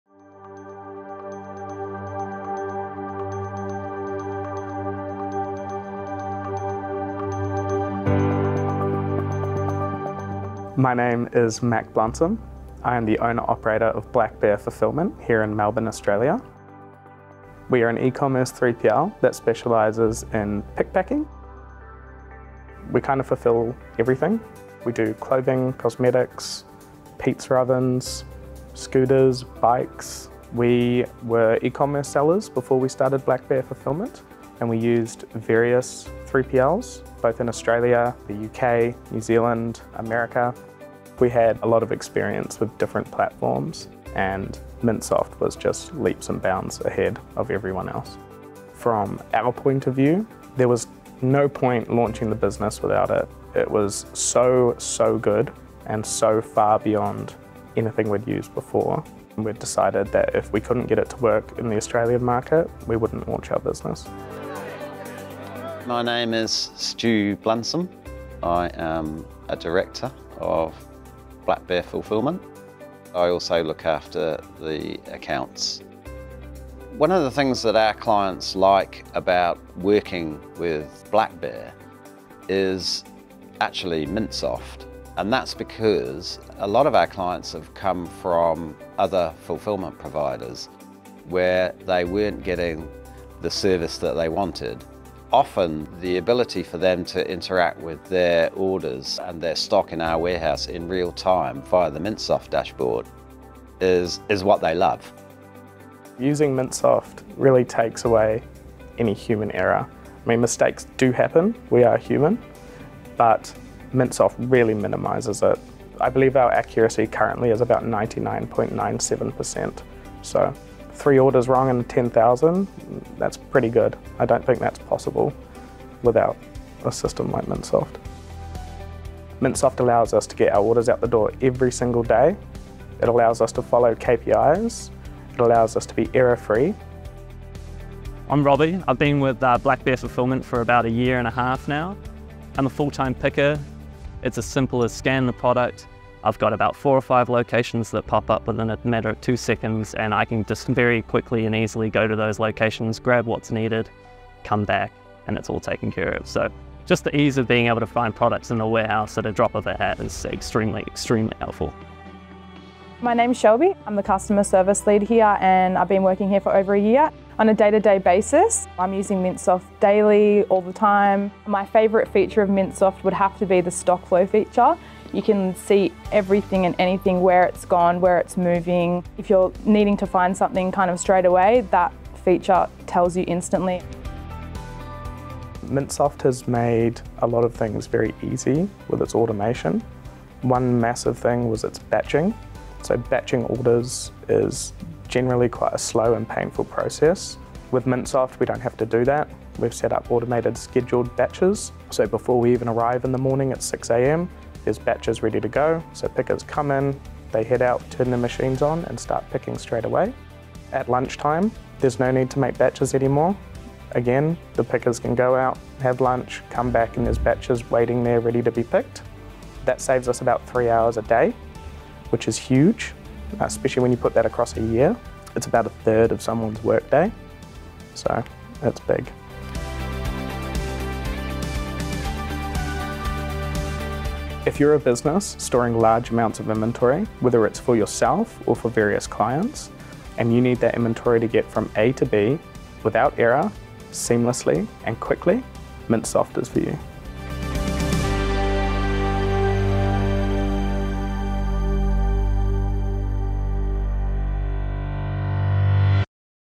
Customer Story - Black Bear Fulfillment - Access Mintsoft
Black Bear Fulfillment are a 3PL, based in Australia providing ecommerce order fulfilment services. They use Access Mintsoft as their 3PL warehouse management software to support their warehouse and ecommerce fulfilment operations.